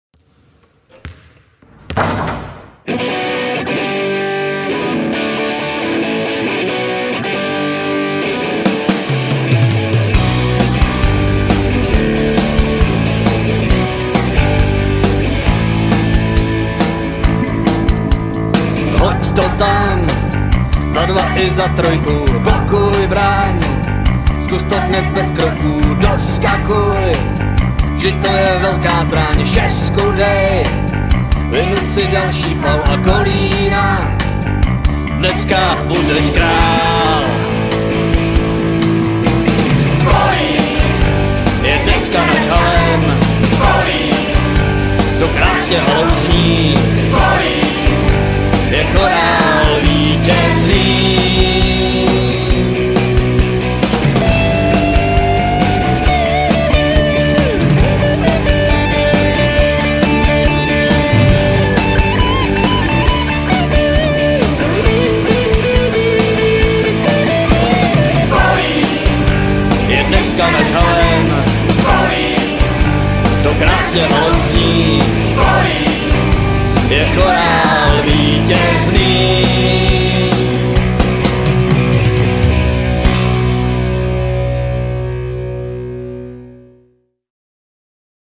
TOP 5 kolínských Medvědů z 1. - 3. kola Kooperativa NBL 2021/2022
Podívejte se na nejlepší akce našich hráčů z prvních 3 kol Kooperativa NBL 2021/2022 a navštivte náš FB profil, kde můžete o nejlepší akci rozhodnout právě Vy a získat věcné ceny věnované naším klubem!